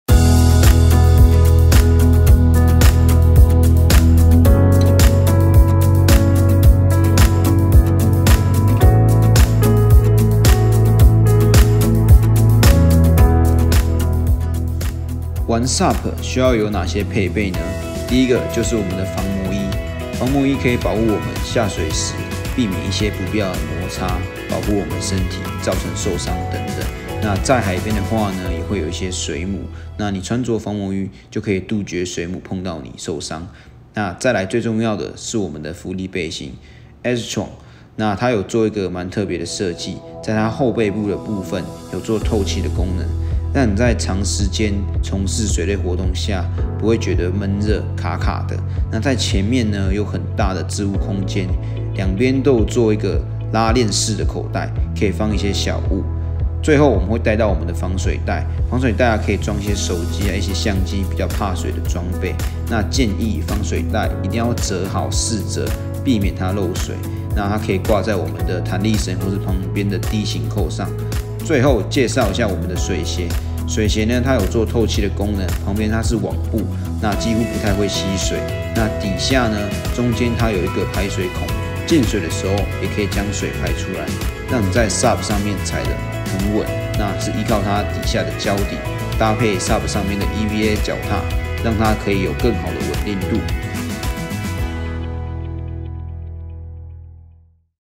【SUP立式划槳小百科系列】玩SUP要帶什麼呢 ? 必需帶的裝備指南 / SUP裝備指南 /一起去划SUP吧 /SUP教學
安心玩SUP 身上需要準備那些配件呢?
浮力背心、防磨衣、防水袋.
城市綠洲都幫你準備好，快來跟綠洲小編Billy一起玩SUP